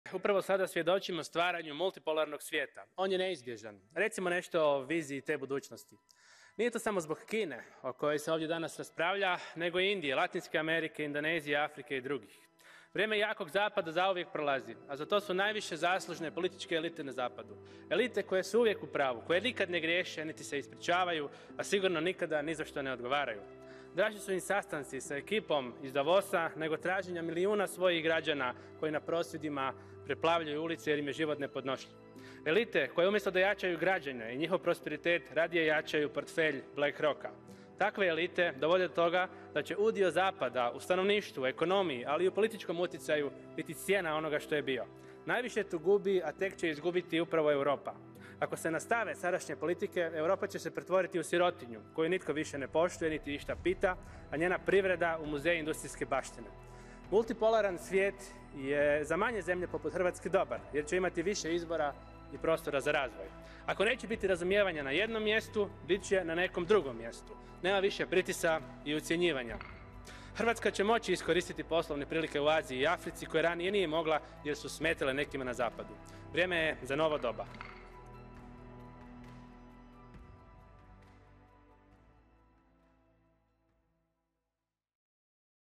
// Evo zašto je multipolaran svijet dobar za Hrvatsku //
Upravo sada svjedočimo stvaranju multipolarnog svijeta. On je neizbježan. Vrijeme jakog Zapada zauvijek prolazi, a za to su najviše zaslužne političke elite na Zapadu.

Multipolaran svijet je za manje zemlje poput Hrvatske dobar jer će imati više izbora i prostora za razvoj.
Ako neće biti razumijevanja na jednom mjestu, bit će na nekom drugom mjestu - nema više pritisaka i ucjenjivanja.

Vrijeme je za novo doba.

// The sole liability for the opinions stated rests with the author. These opinions do not necessarily reflect the opinions of the European Parliament. //